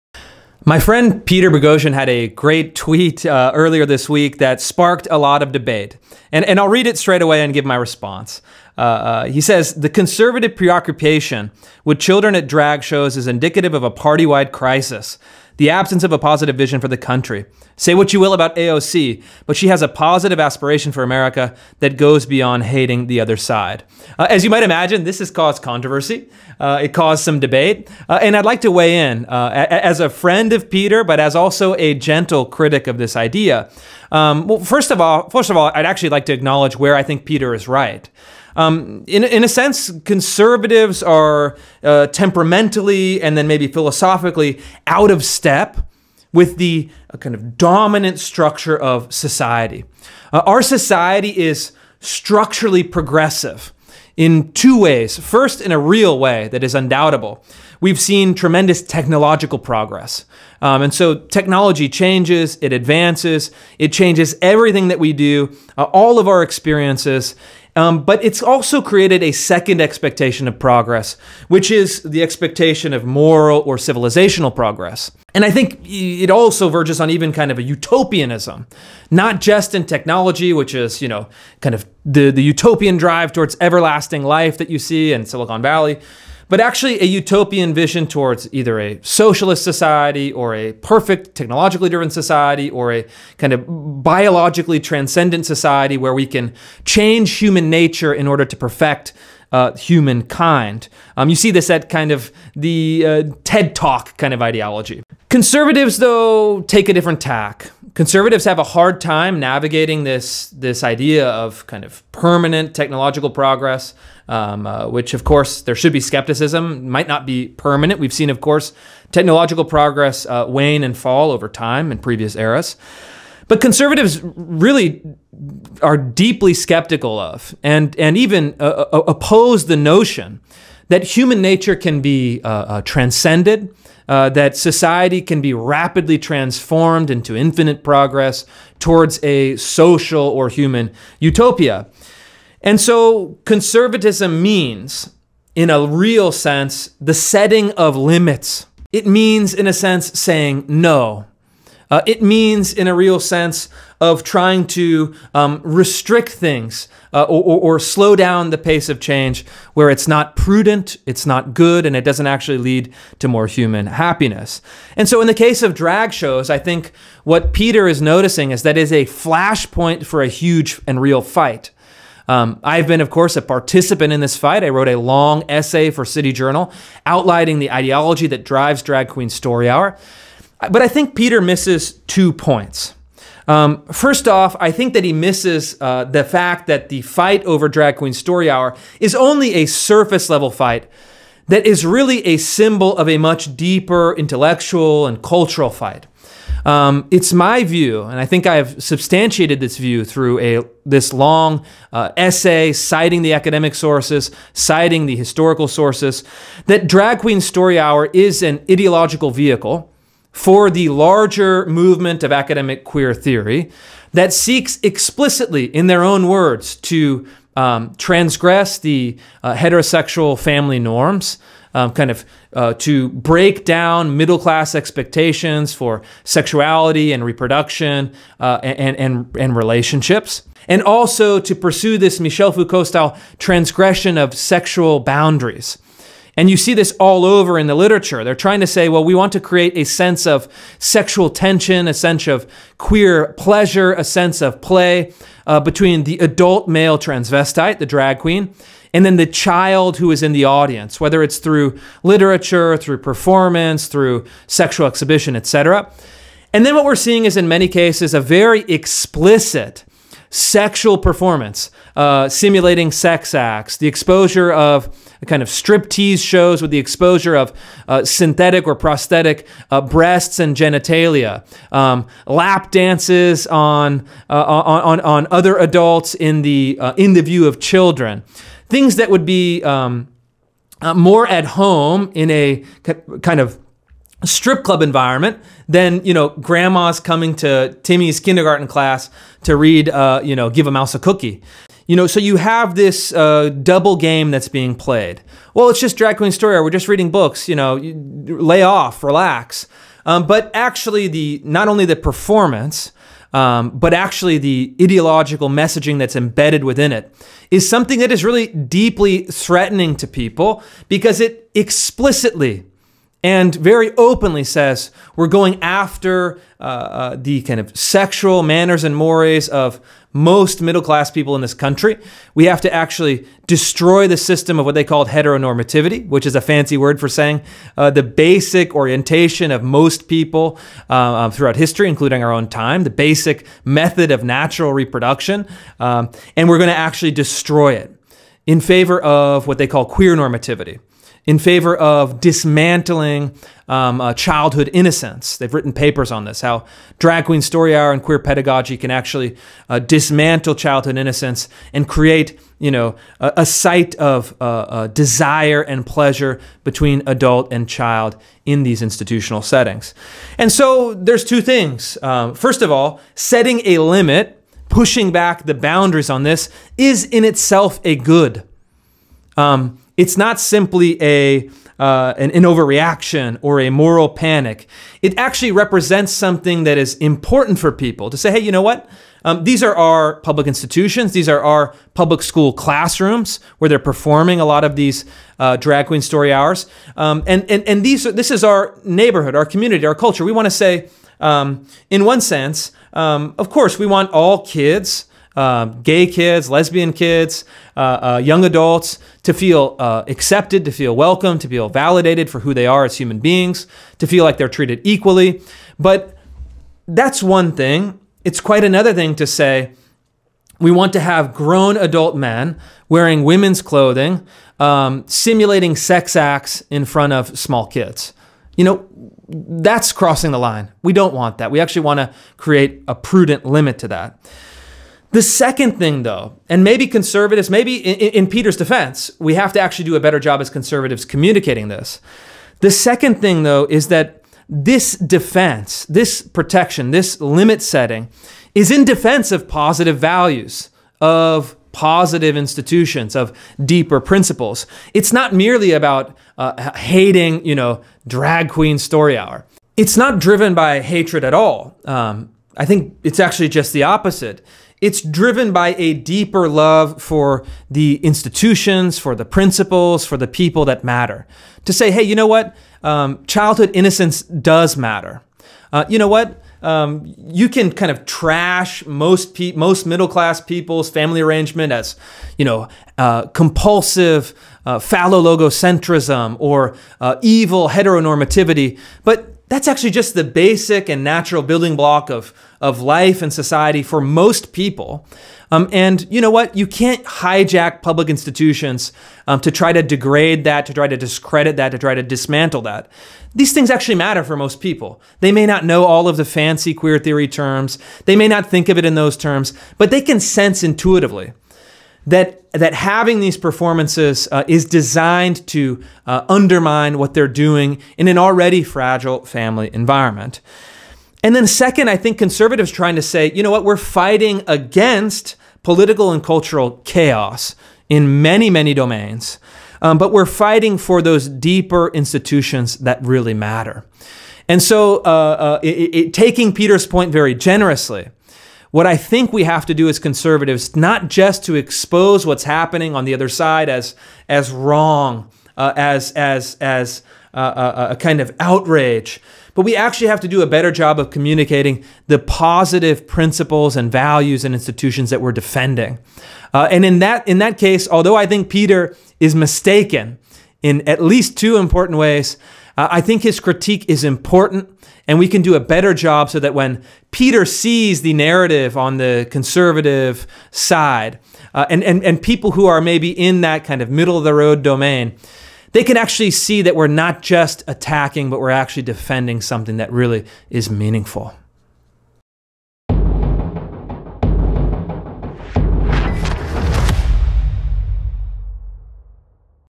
Why Conservatives Oppose Drag Queen Story Hour
I respond to Peter Boghossian’s criticism that conservatives focus on the culture war because they lack a positive vision.